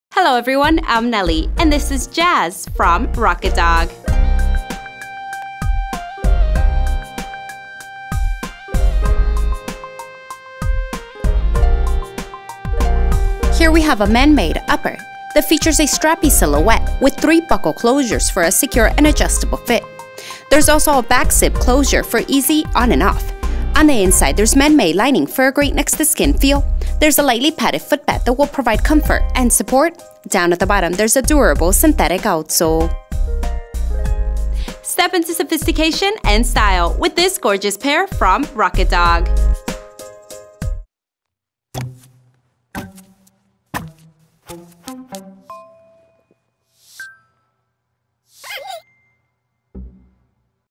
Rocket Dog Jaz SKU:8753149
Paint the town in the Rocket Dog® Jaz flats!
Man-made upper features a strappy silhouette.
Zip-up closure with buckled accents.
Pointed toe.
Man-made lining.
Lightly-padded insole.
Synthetic outsole.
Imported.